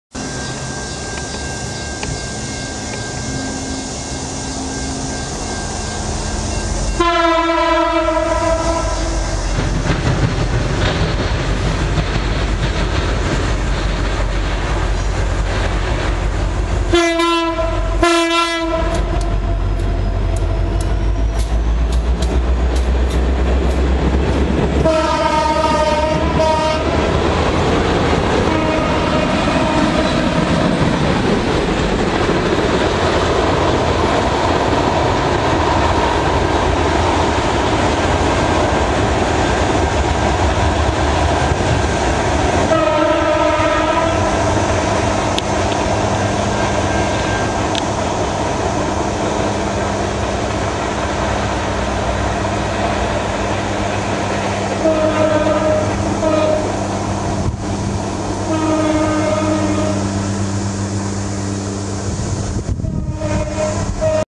A Lost Clip from Railfanning Rochelle with a weird BNSF
Recommended to be seen in 480p! As you can see this lost clip that came from my old camera has a short BNSF with the funniest horn i have heard. DO NOT WORRY MY QUALITY ON ALL OTHER VIDEOS WILL BE 1080p THIS IS JUST AN OLDER CLIP